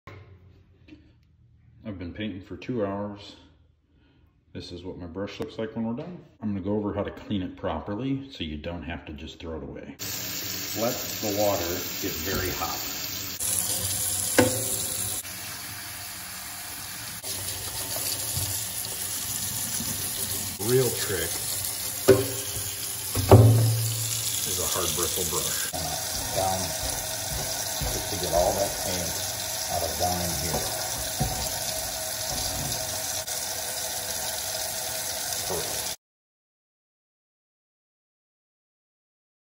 [Daily Tip] How-To Clean Your Paint Brush!
Are you sick of throwing away paint brushes because they get hard? Follow these steps! Straight to the point, no B.S.